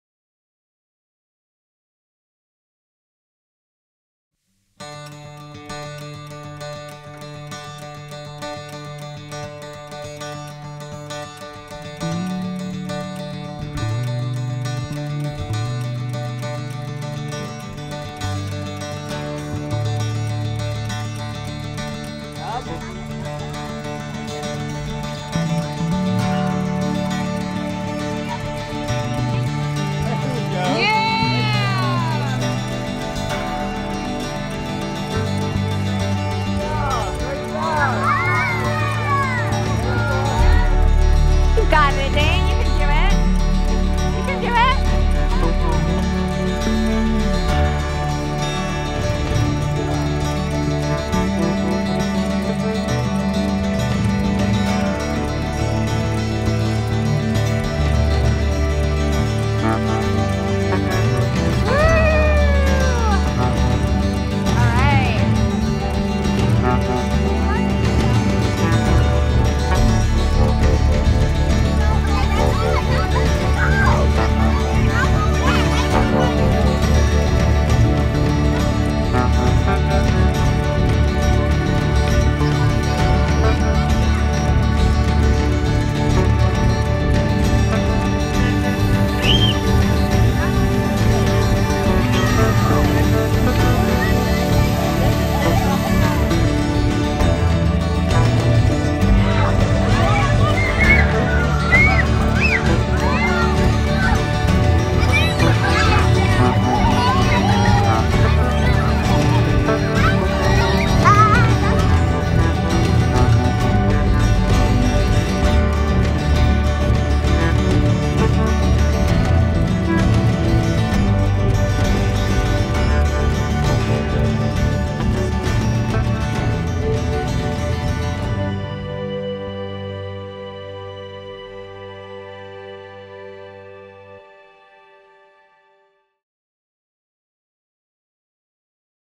Welcome to Middle School, Grade 6!
At the start of their sixth grade year, students at WSA enter middle school with a flourish! Check out their recent adventure...